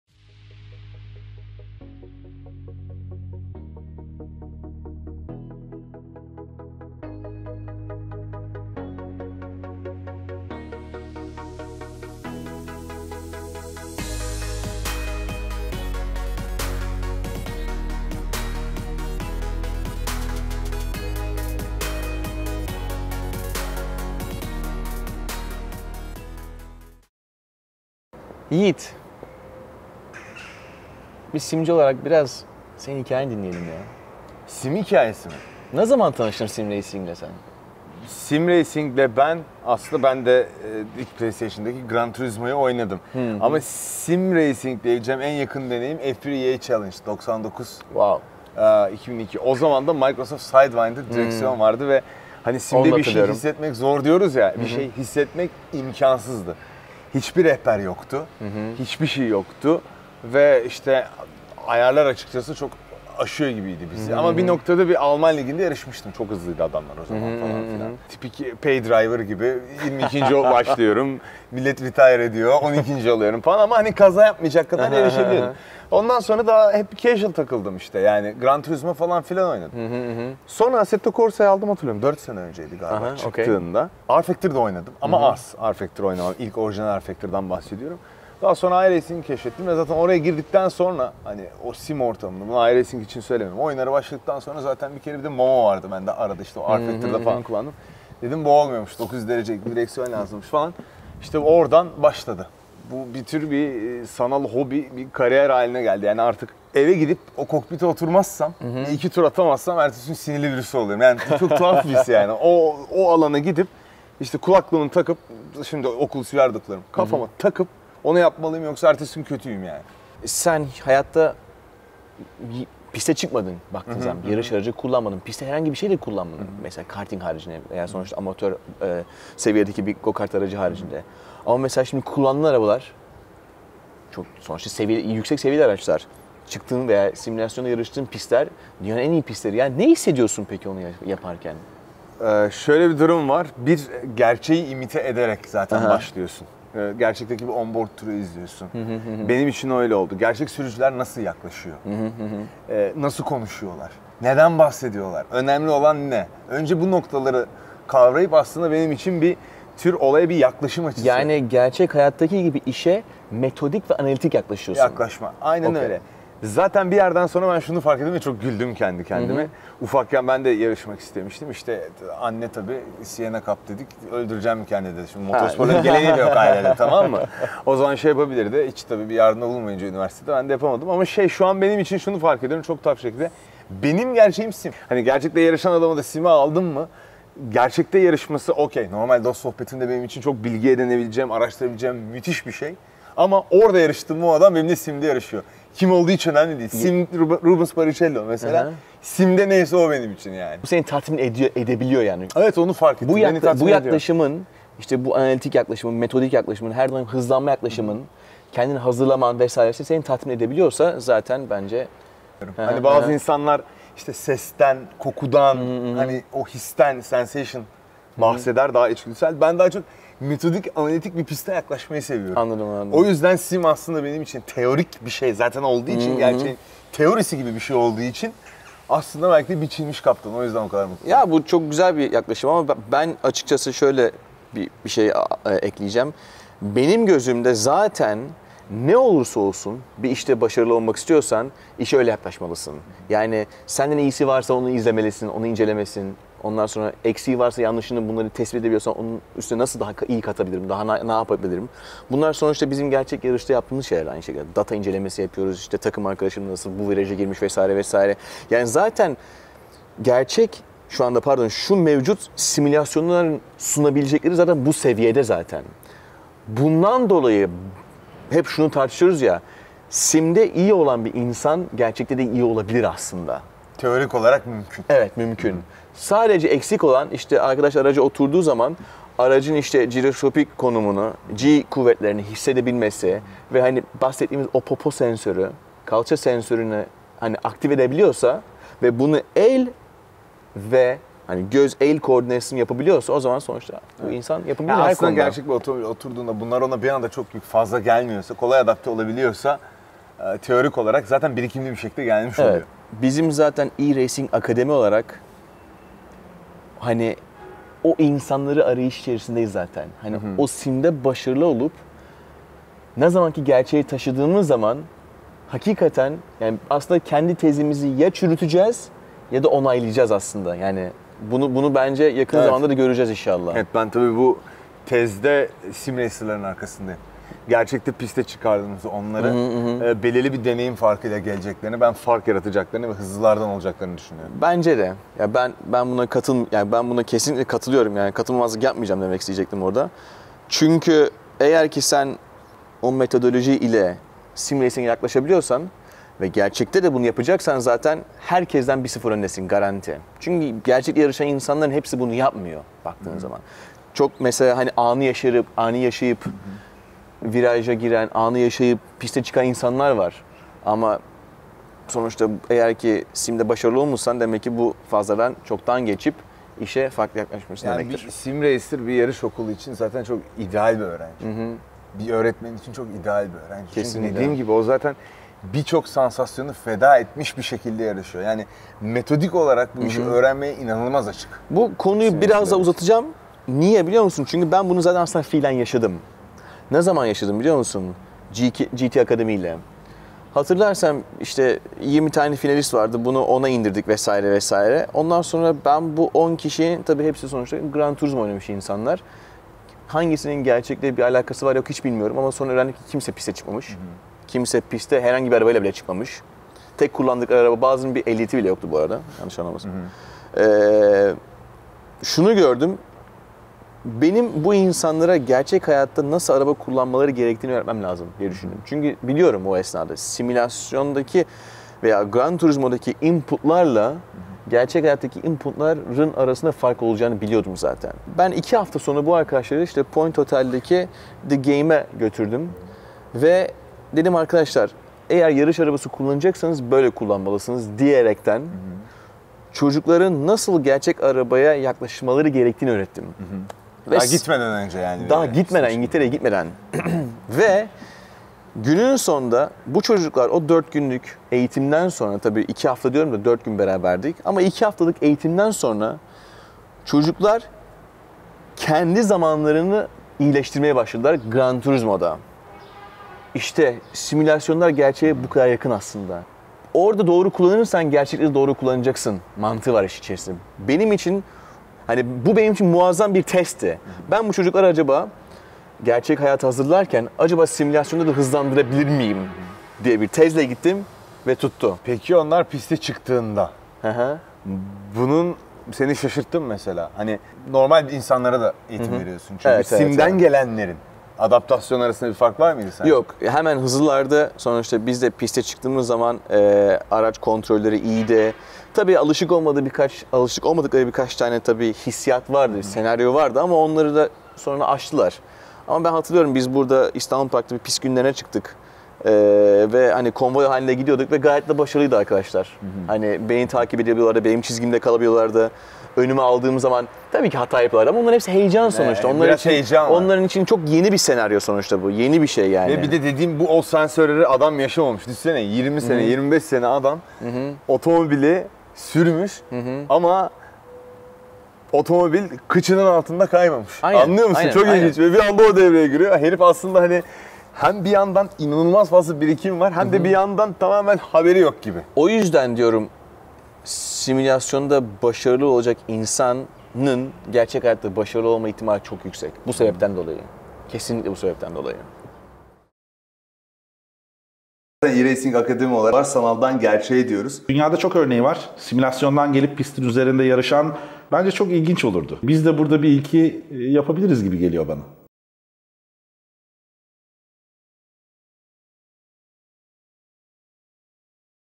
eRacing Academy / SimRacing Muhabbeti
Jason Tahincioğlu ve Yiğit Tezcan, İntercity İstanbul Park'ta simülasyon ve gerçek yarışlar üzerine, daha önce pek görülmemiş türden bir sohbet yaptı.

Bu video'da gerçek bir yarış sürücüsüyle bir sim yarışçısının bakış açılarının nerelerde benzeşip, nerelerde ayrıldığını gözlemleyebilir ve eRacing Academy olarak, bu işe nasıl baktığımızı daha iyi anlayabilirsin diye düşünüyoruz.

İyi seyirler!